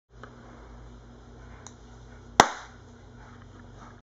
200mw Ebay Laser Popping A Balloon
Got It for 40 bucks